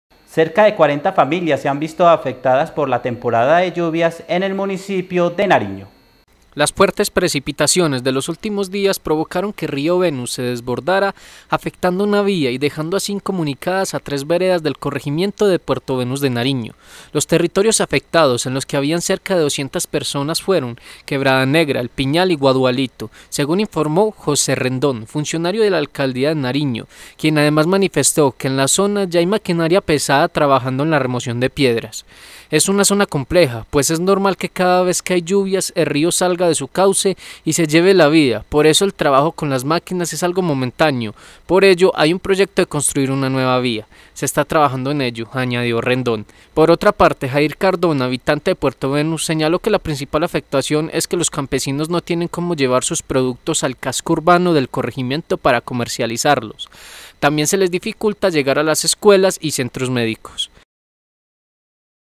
TRES VEREDAS AFECTADAS POR NUEVO DESBORDAMIENTO EN PUERTO VENUS
Cerca de 40 familias han resultado afectadas por la temporada de lluvias en el municipio de Nariño.